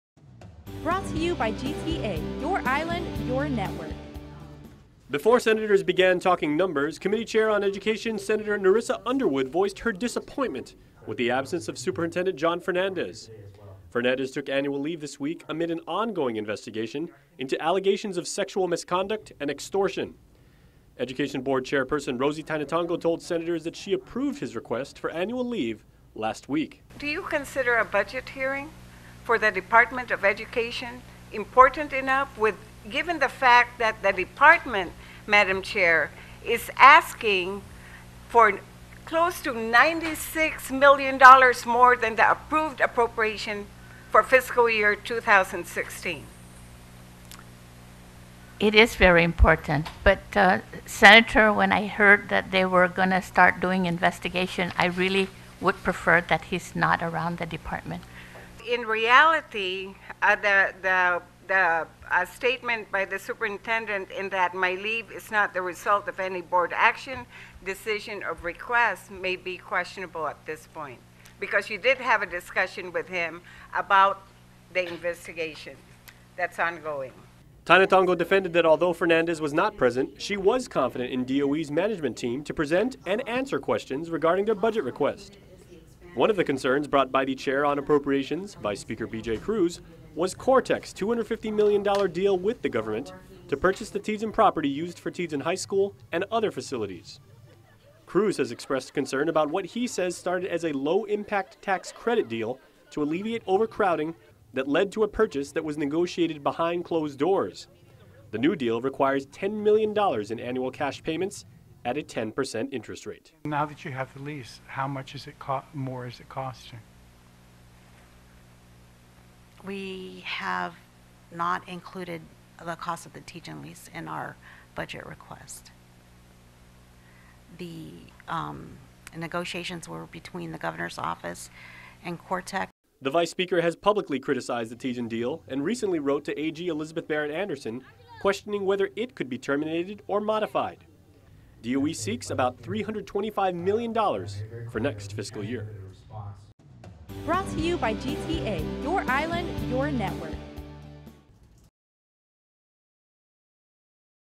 DOE seeks budget of $325 million for Fiscal Year 2017
Before senators began talking numbers, chairperson of the Committee on Education Senator Nerissa Underwood voiced her disappointment with the absence of superintendent Jon Fernandez. Fernandez took annual leave this week amid an ongoing investigation into allegations of sexual misconduct and extortion.